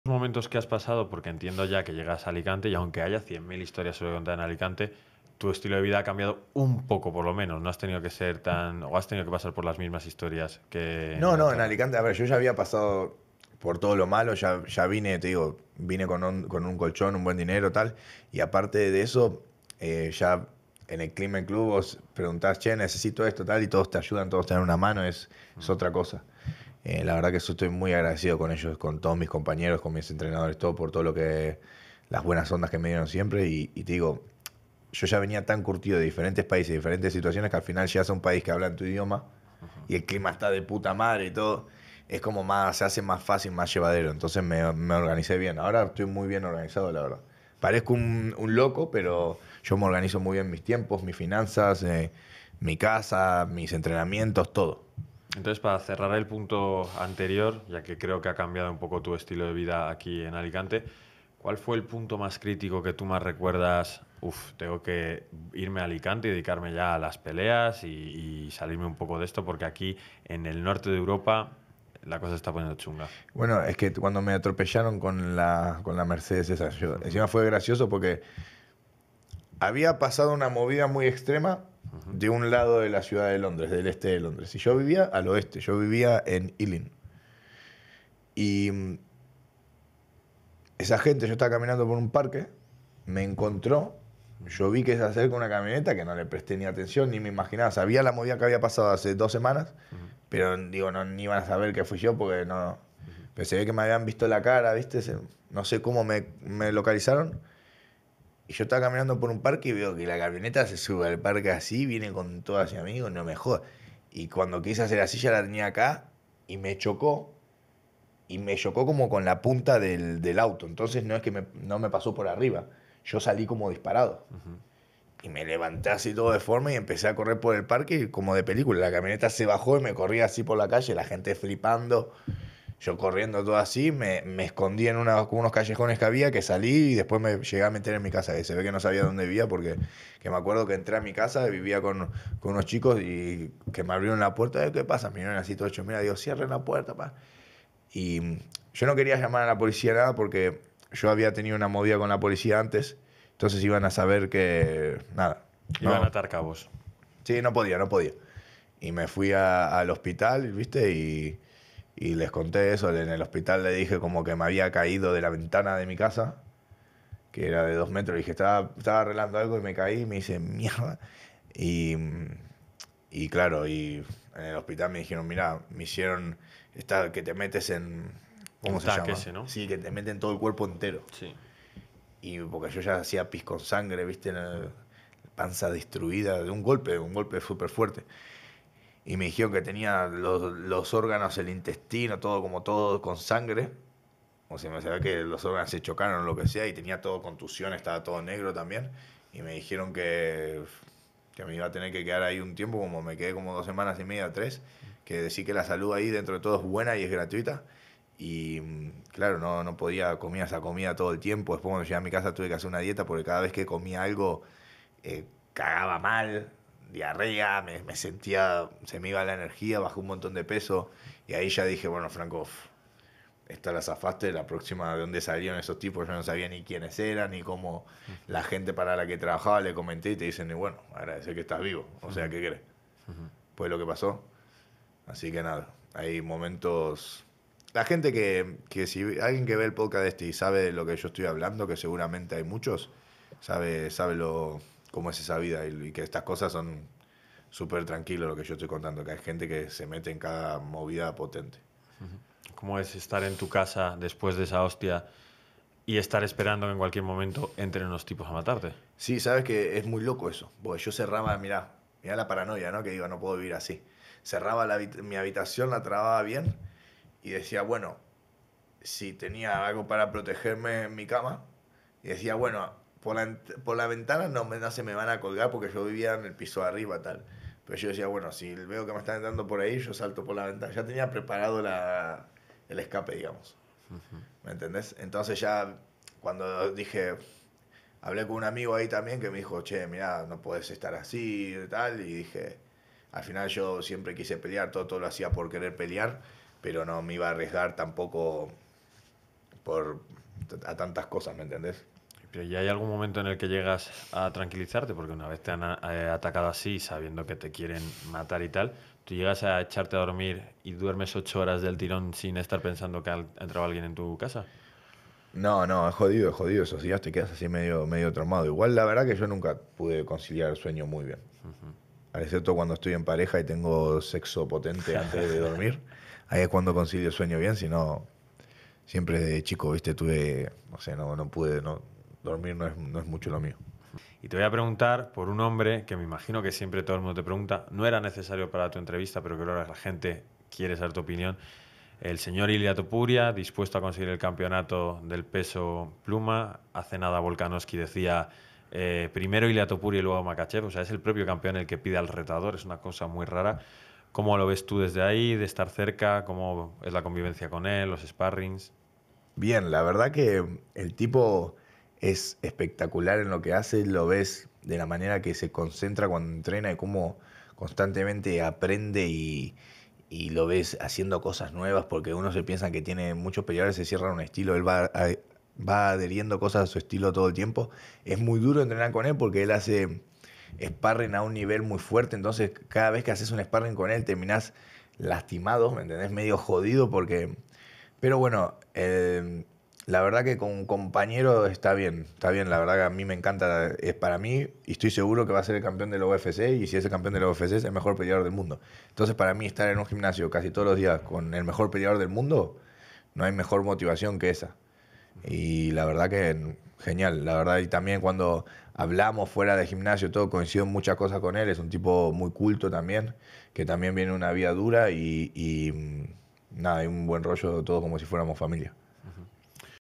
El REY de la CALLE opina sobre ILIA TOPURIA
Hoy en Generación RAW Gonzalo pregunta a Franco Tenaglia sobre el momento concreto que le hizo cambiar de vida, a lo que el peleador argentino responde que un intento de asesinato, concretamente un atropello. Además Franco nos da su opinión sobre Ilia Topuria, quien entrena con él en el Climent.

Si quieres participar, proponer ideas o patrocinar el programa ponte en contacto